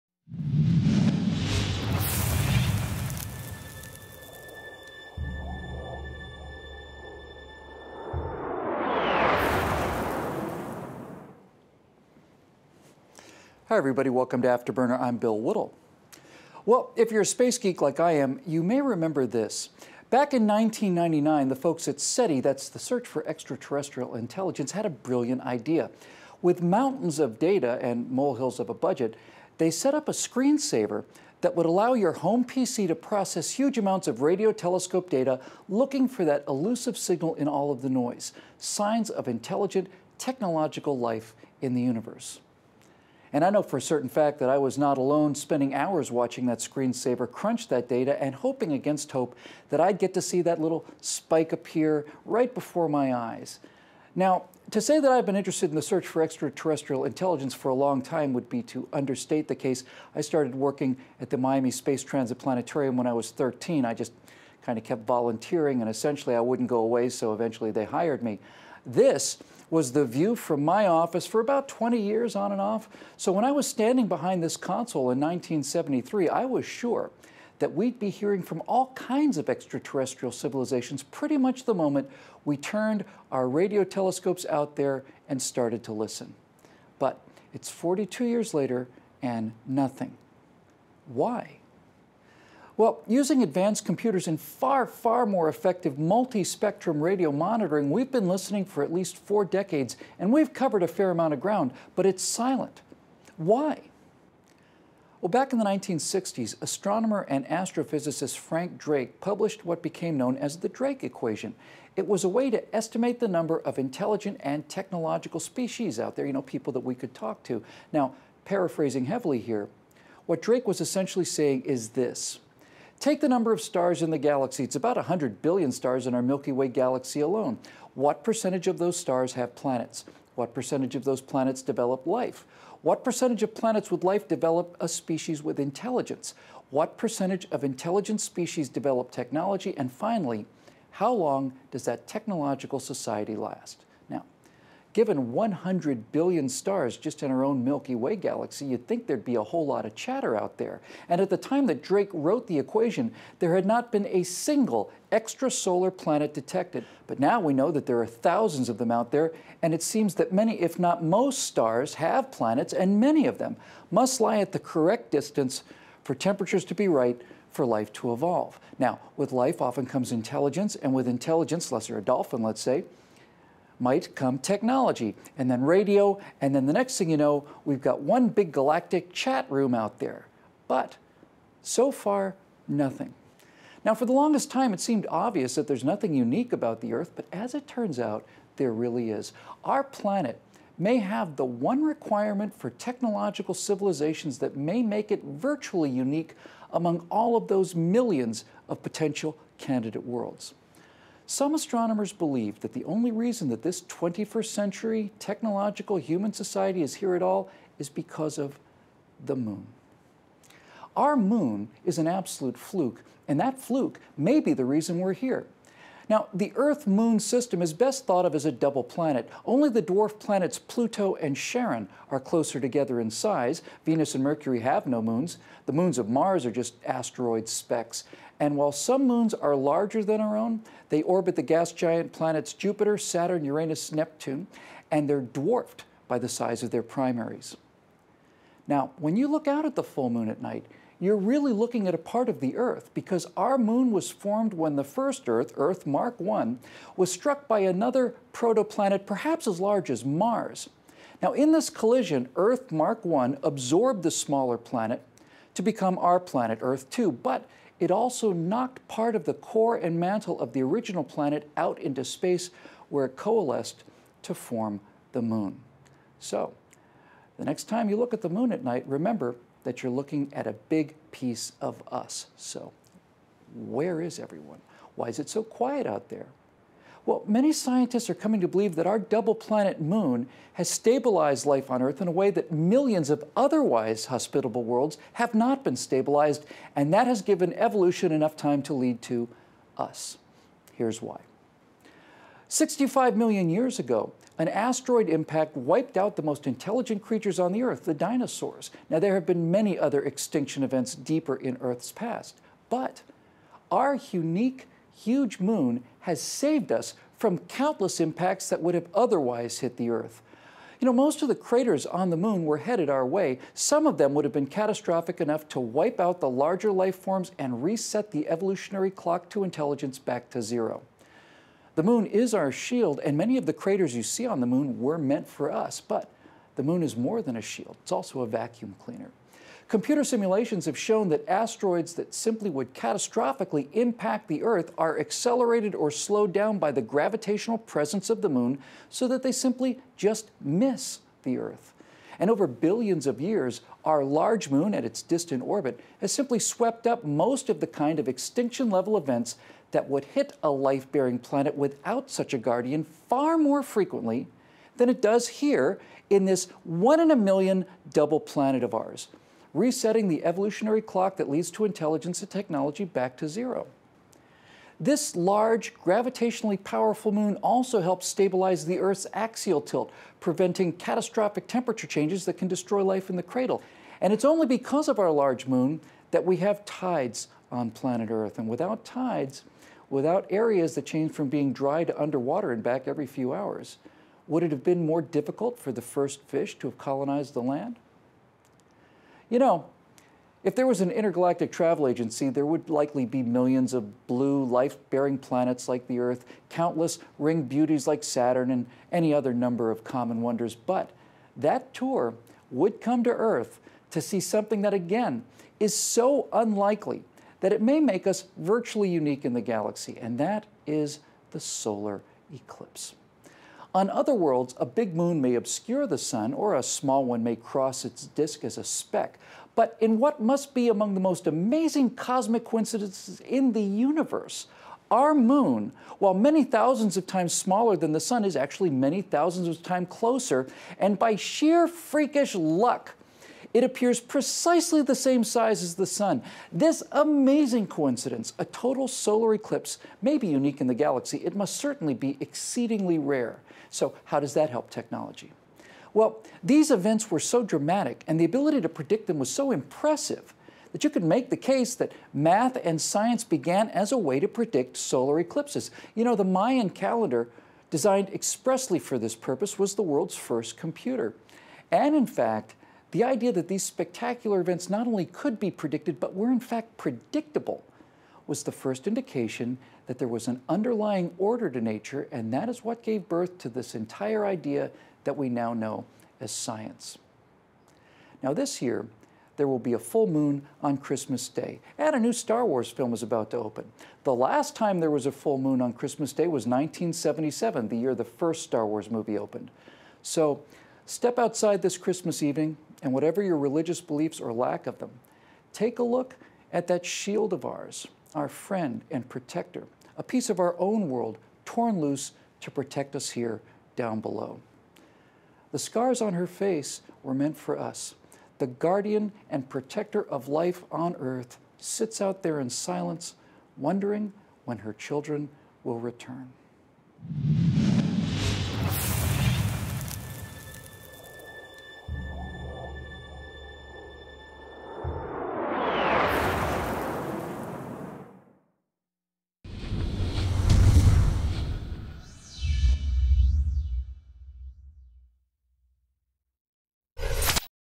Where is Everybody? Why Haven't We Found Extra-Terrestrials?
With millions of solar systems in our galaxy, you would think we would have found extra-terrestrial intelligent lifeforms by now... so why haven't we?